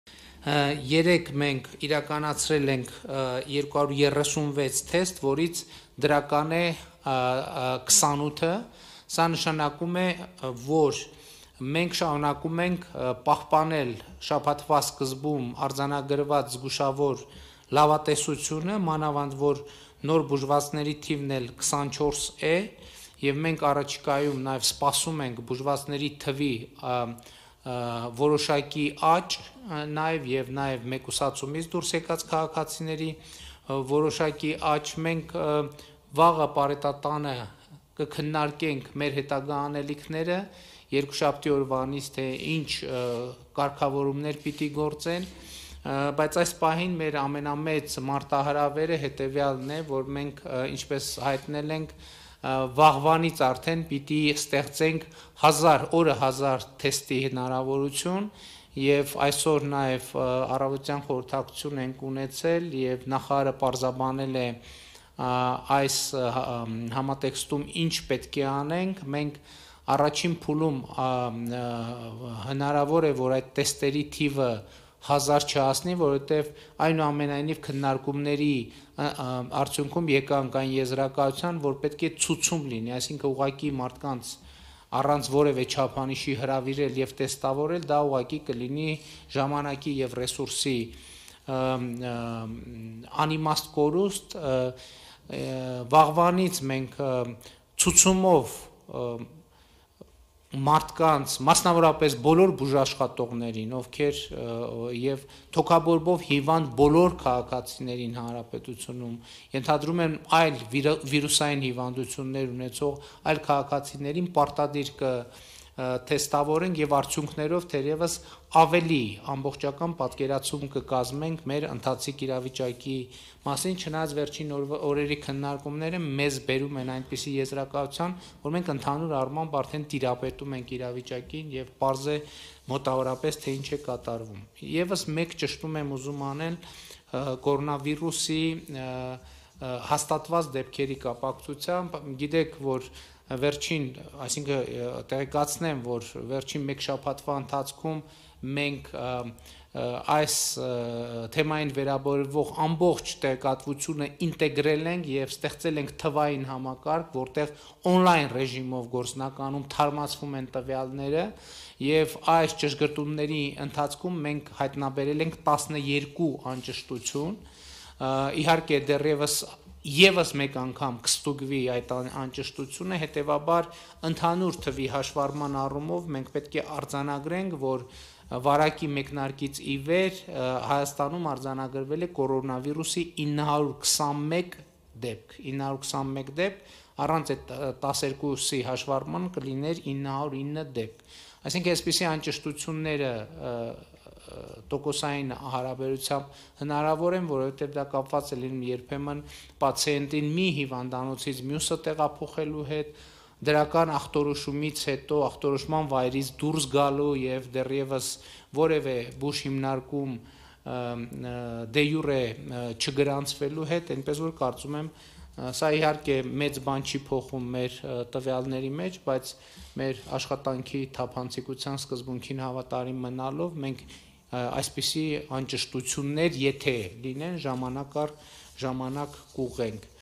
Հաստատված 28 նոր դեպք, 12 անճշտություն, 24 ապաքինված, ընդհանուր՝ 921. Փաշինյան
Հայաստանում կորոնավիրուսի վարակի հաստատված 28 նոր դեպք, 12 անճշտություն, 24 ապաքինված, ընդհանուր՝ 921. Փաշինյան

Վաղվանից բոլոր ցուցումով հիվանդները և բուժաշխատողները պարտադիր կթեստավորվեն. Փաշինյան